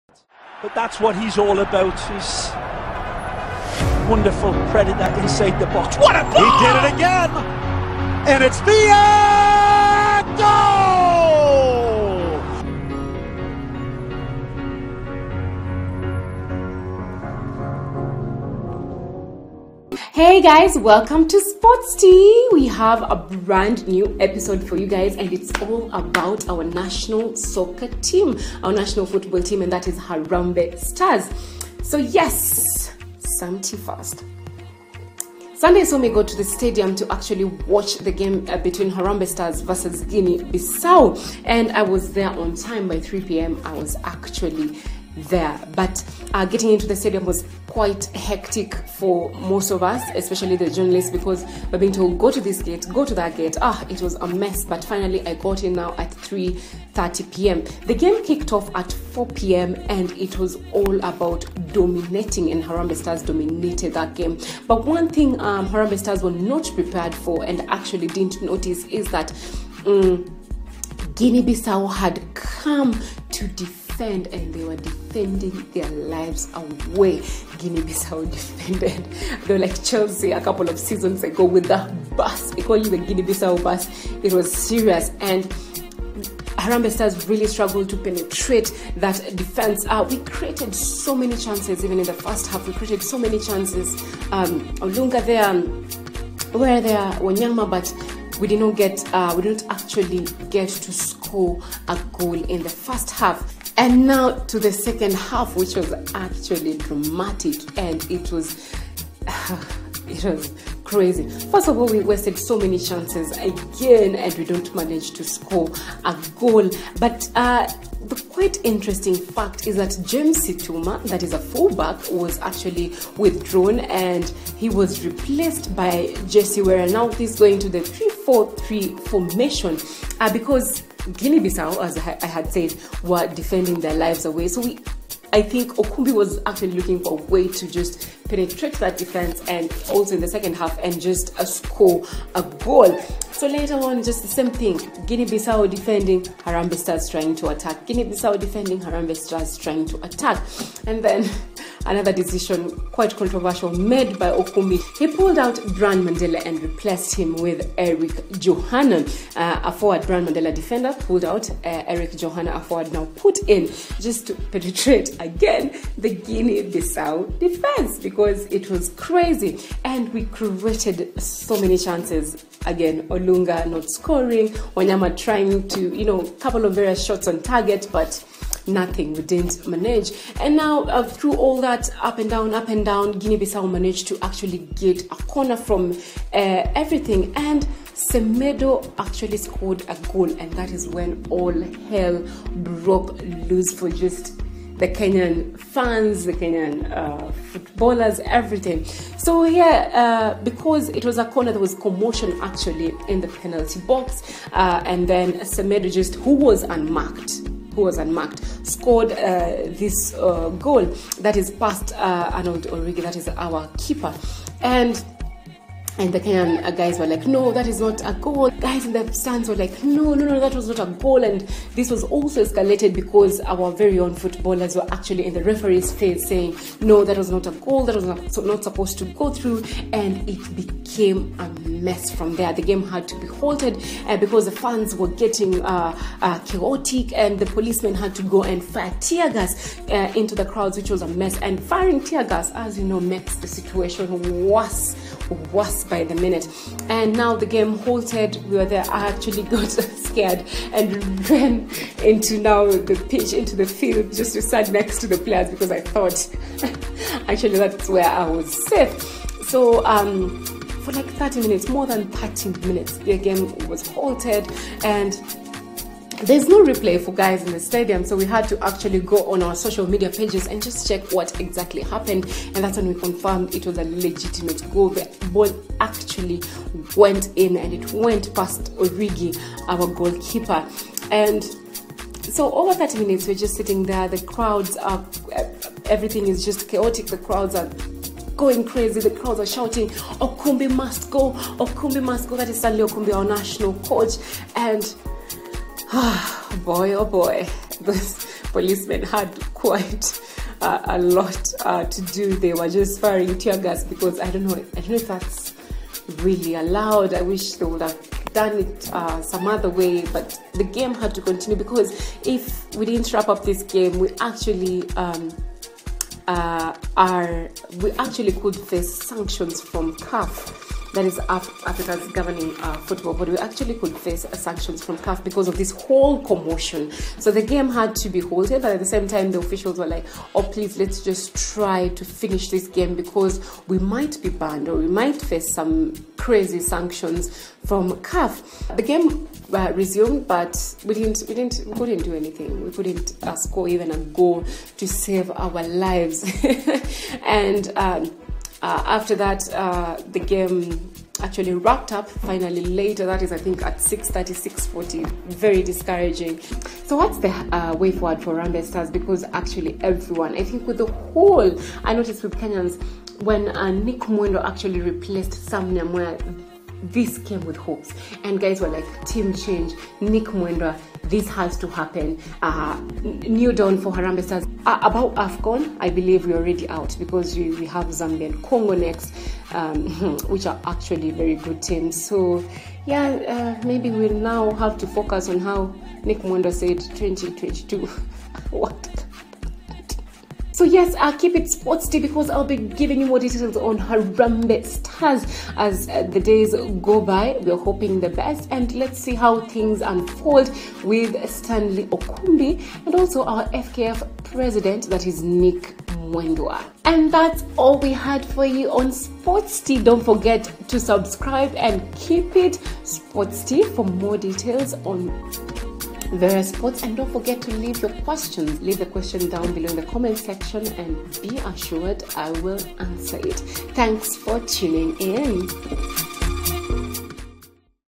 SHAWN OSIMBO | SPORTS TEA [ HARAMBEE STARS' WOES ]
On This Week's Episode Of Sports Tea, We Focus On Harambee Stars' Misfortunes In Their Quest To Qualify For The African Cup Of Nations. Will They Make It Through?